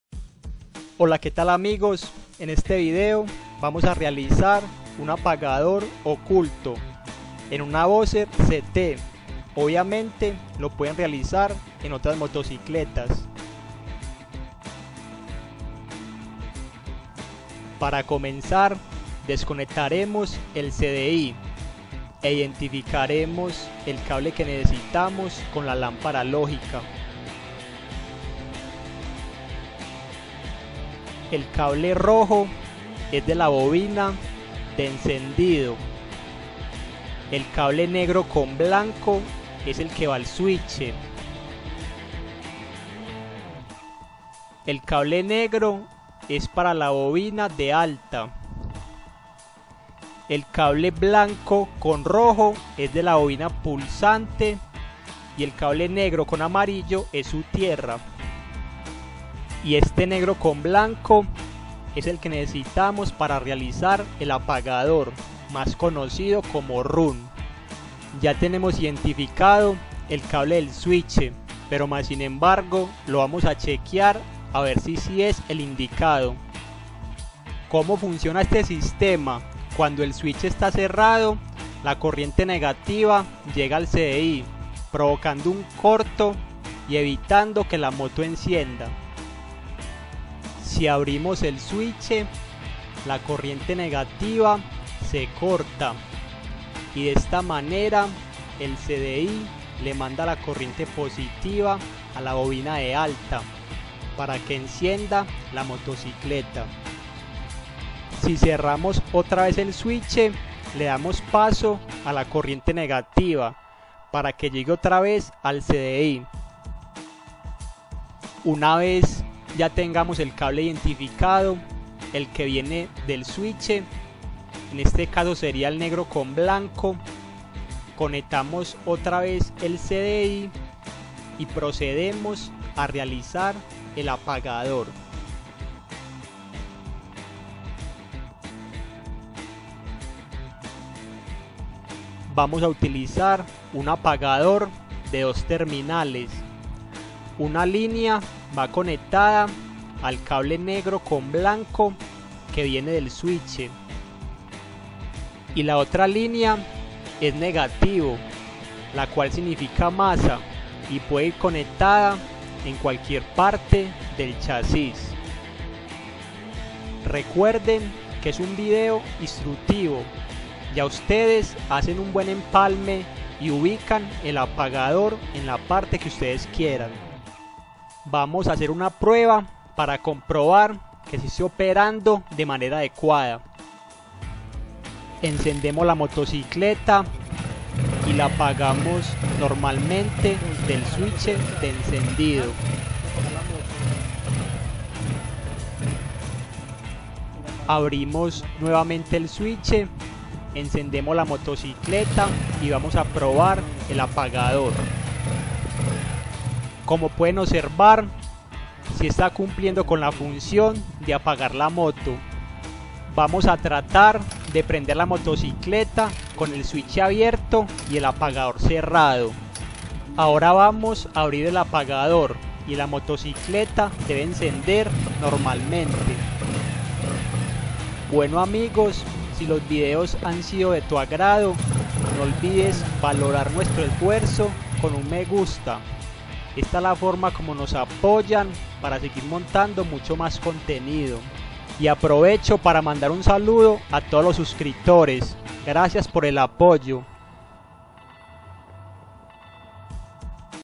Como Evitar El Robo De Tu Moto, Apagador Oculto Boxer CT 100
Resuelve Cualquier *Falla* en el Carburador con este Método *Exclusivo* Run, Apagador Oculto Para Evitar Que Te Roben La Motocicleta Escapeada, Boxer CT 100